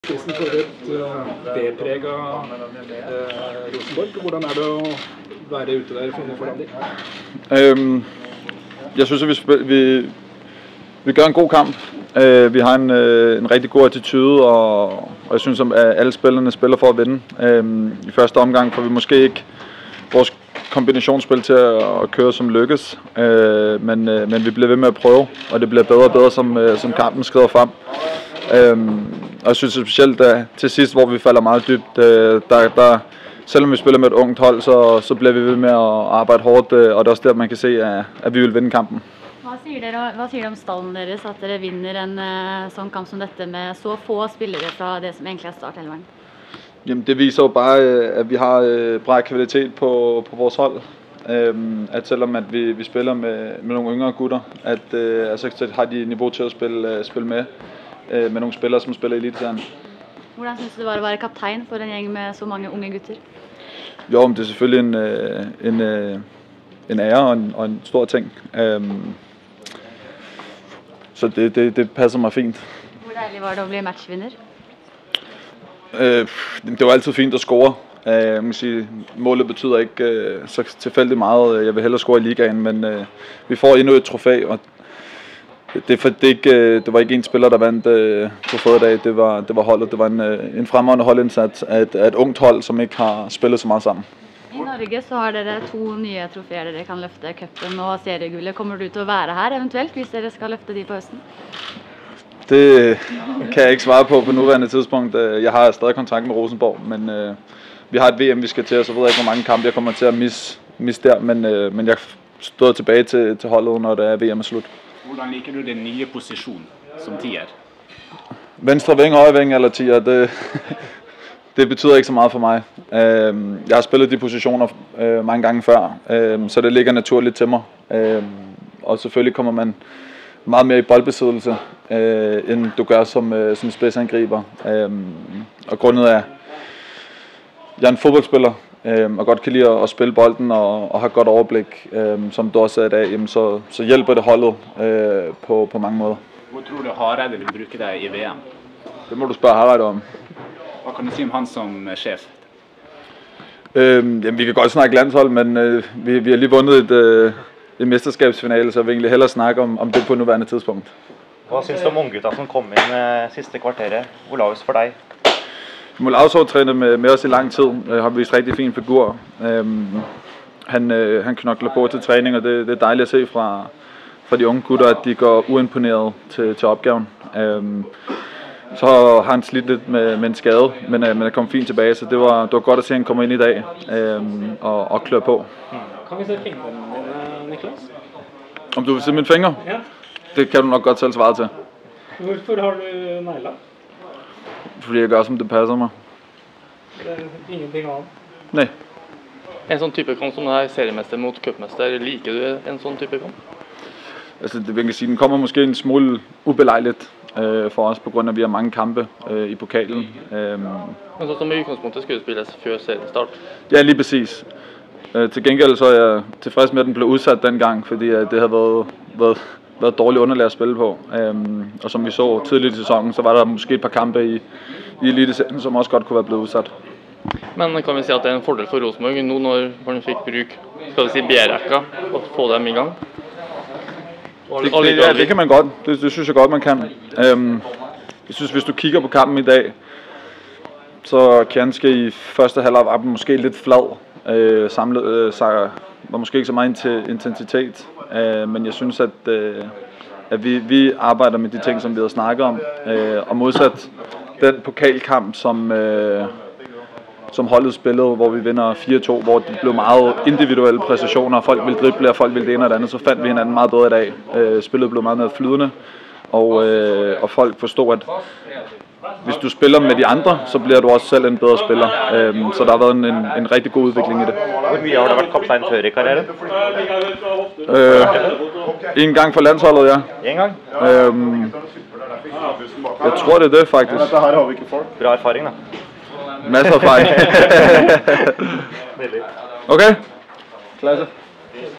RBK Nicklas Bendtner etter LSK 26.4.2018
Intervju med Nicklas Bendtner etter mesterfinalen mot LSK på Åråsen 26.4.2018,